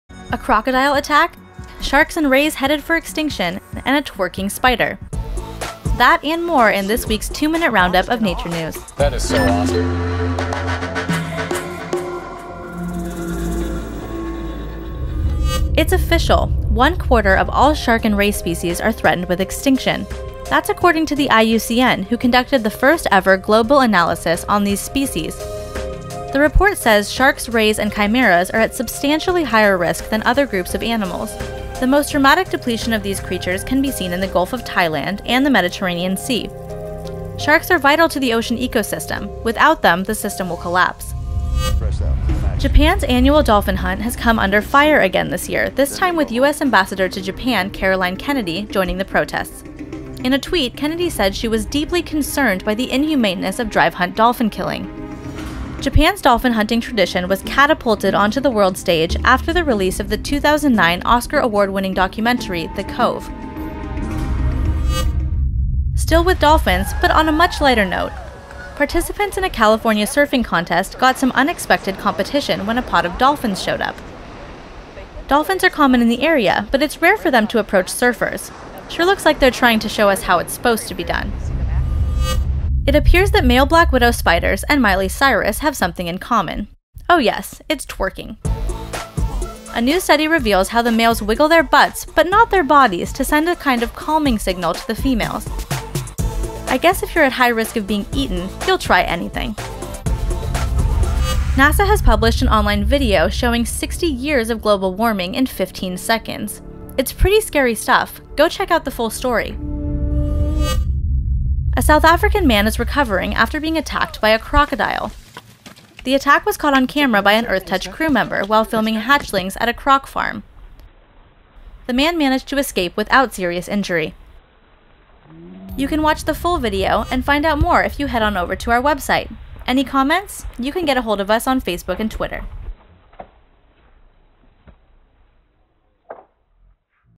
Surf's up, twerking spiders & a croc attack | Nature News Roundup 01-24-2014
Dolphins show surfers how it's done, a little twerking helps male spiders woo, bad news for the world's sharks and a croc attack one farmer didn't see coming. All those stories and more in our weekly roundup of nature news!